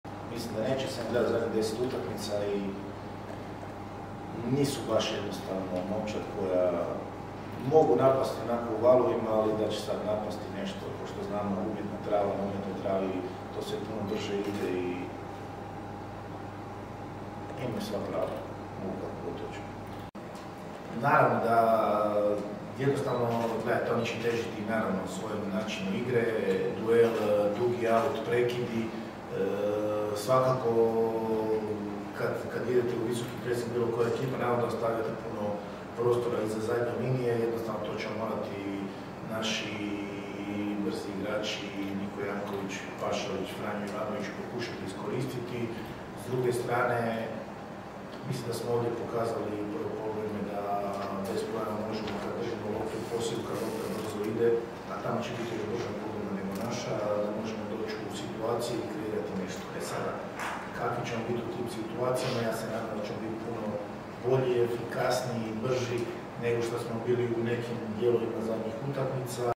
Nakon presice Sopića uoči utakmice sa švedskim Elfsborgom, Sopiću je uručen otkaz
Nakon presice Sopića uoči utakmice sa švedskim Elfsborgom, Sopić je dobio otkaz